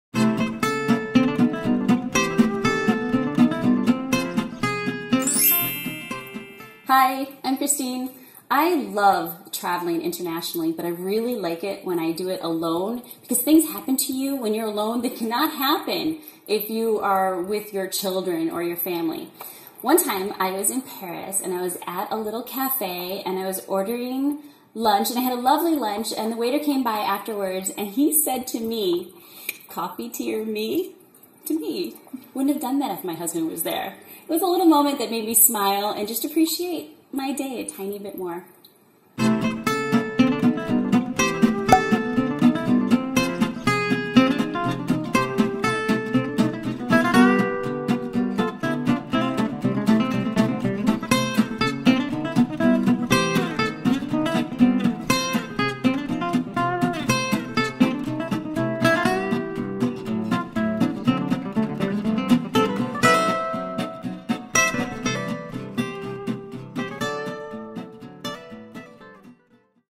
Benefits of Traveling Alone: Little Moments by Kristine Castagnaro
Hi. My name is Kristine Castagnaro and I love traveling internationally but I really like it when I travel alone because things happen to you when you are alone that can't happen if you are with your children or your family. One time I was in Paris and I was at a little cafe and I was ordering lunch. I had a lovely lunch and the waiter came by afterwards and he said to me, "Coffee, tea, or me?" To me! He wouldn't have done that if my husband was there. (I chose the tea, of course! What were YOU thinking?) It was a little moment that made me smile and appreciate my day a tiny bit more.